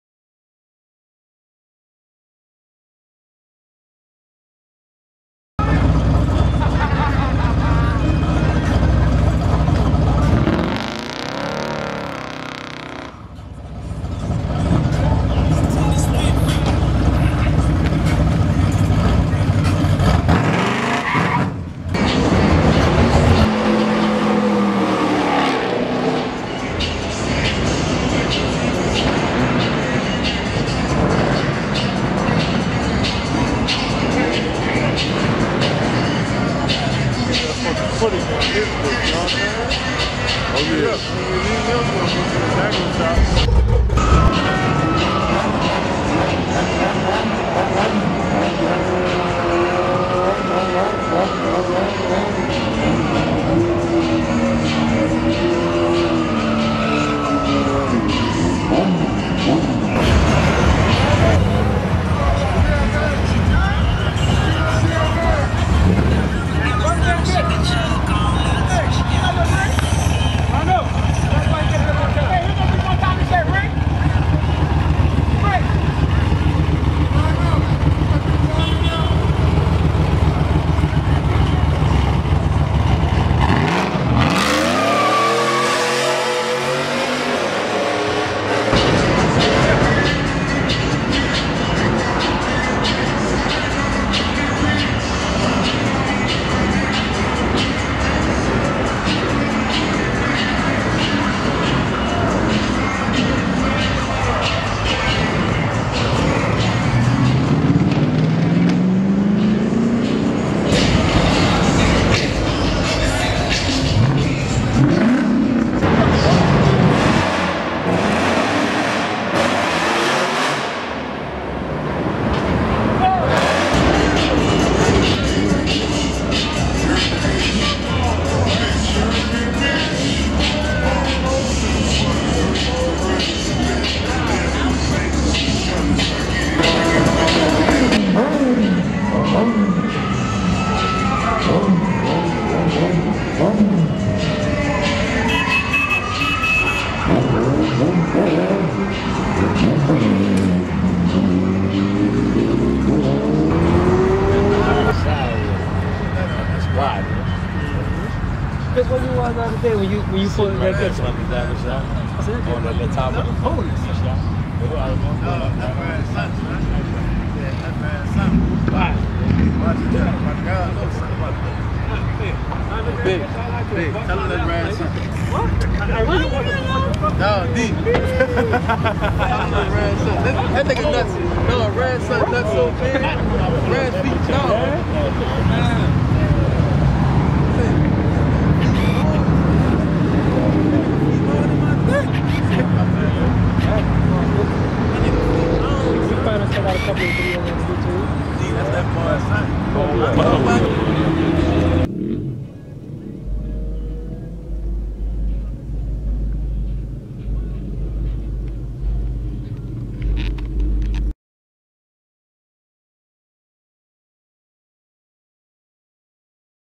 Veltboy314 - First time chilling at 2nd line in New Orleans
First time chilling at 2nd line in New Orleans